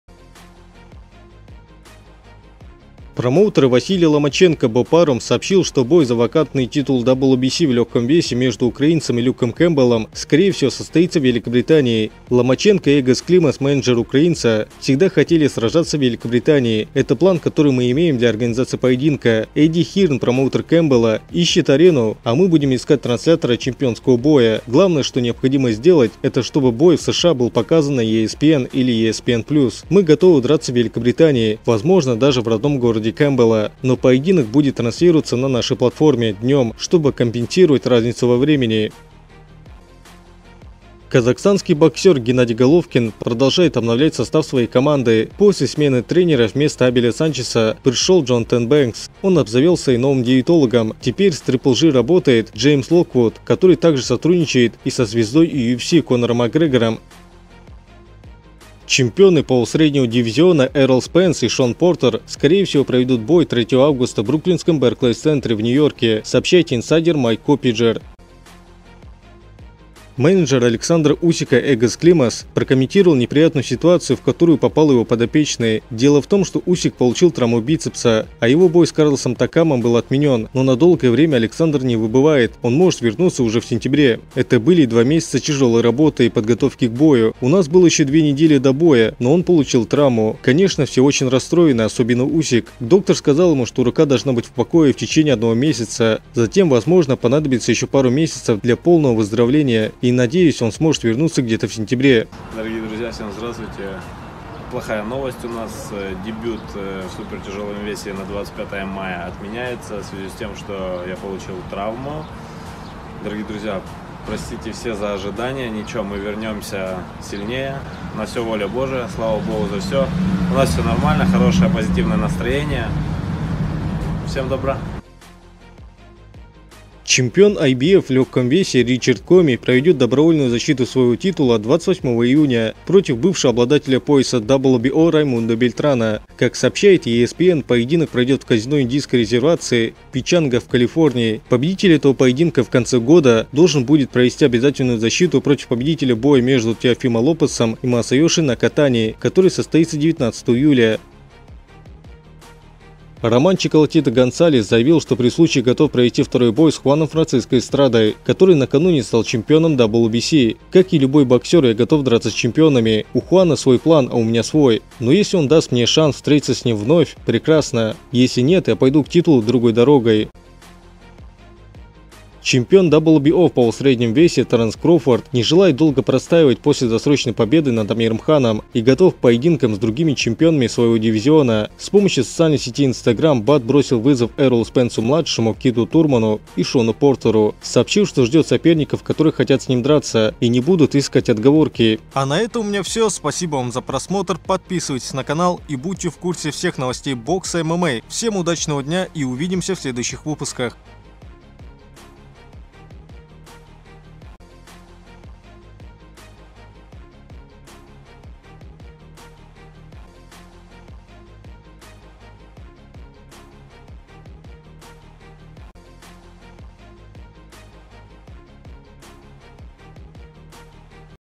Головкин нанял специалиста из команды Макгрегора/ Простите! Усик обратился к фанатам!
Чемпион IBF в легком весе Ричард Комми проведет добровольную защиту своего титула 28 июня против бывшего обладателя пояса WBO Раймундо Бельтрана. Как сообщает ESPN, поединок пройдет в казино индейской резервации Печанга в Калифорнии.
Победитель этого поединка в конце года должен будет провести обязательную защиту против победителя боя между Теофимо Лопесом и Масайоши Накатани, который состоится 19 июля.

Менеджер Александра Усика Эгис Климас прокомментировал неприятную ситуацию, в которую попал его подопечный. Дело в том, что Усик получил травму бицепса, и его бой с Карлосом Такамом был отменён, но на долгое время Александр не выбывает, он может вернуться уже в сентябре.
«Это были два месяца тяжёлой работы и подготовки к бою. У нас было ещё две недели до боя, но он получил травму. Конечно, все очень расстроены, особенно Усик. Доктор сказал ему, что рука должна быть в покое в течение одного месяца. Затем, возможно, понадобится ещё пару месяцев для полного выздоровления и, надеюсь, он сможет вернуться где-то в сентябре», — сказал Климас.

Чемпионы полусреднего дивизиона Эррол Спенс (IBF) и Шон Портер (WBC) скорее всего проведут бой 3 августа в бруклинском Barclay’s Center, Нью-Йорк — сообщает badlefthook.com со ссылкой на инсайдера Майка Коппингера.

Роман Чоколатито Гонсалес заявил, что при случае готов провести второй бой с Хуаном Франсиско Эстрадой, который накануне стал чемпионом WBC…
«Как и любой боксер, я готов драться с чемпионами. У Хуана свои планы, а у меня свои, но если он даст мне шанс встретиться с ним вновь — прекрасно. Если нет, я пойду к титулу другой дорогой.— говорит Чоколатито.

Чемпион WBO в полусреднем весе Теренс Кроуфорд не желает долго простаивать после досрочной победы над Амиром Ханом и готов к поединкам с другими чемпионами своего дивизиона. С помощью социальной сети Instagram "Бад" бросил вызовы Эрролу Спенсу-младшему (владеет поясом IBF), Киту Турману (WBA Super) и Шону Портеру (WBC), сообщив, что ждет соперников, которые хотят с ним драться и не будут искать отговорки.

Промоутер Василия Ломаченко Боб Арум сообщил, что бой за вакантный титул WBC в легком весе между украинцем и Люком Кэмпбеллом, скорее всего, состоится в Великобритании.
«Ломаченко и Эгис Климас, менеджер украинца, всегда хотели сражаться в Великобритании. Это план, который мы имеем для организации поединка. Эдди Хирн, промоутер Кэмпбелла, ищет арену, а мы будем искать транслятора чемпионского боя.
Главное, что необходимо сделать, это чтобы бой в США был показан на ESPN или ESPN+. Мы готовы драться в Великобритании, возможно, даже в родном городе Кэмпбелла. Но поединок будет транслироваться на нашей платформе днем, чтобы компенсировать разницу во времени», — передает слова Арума портал.

Казахстанский боксер Геннадий Головкин продолжил обновлять состав своей команды
После смены тренера — вместо Абеля Санчеса пришел Джонатон Бэнкс — он обзавелся и новым диетологом: теперь с GGG работает Джеймс Локвуд, который также сотрудничает и со звездой UFC Конором Макгрегором.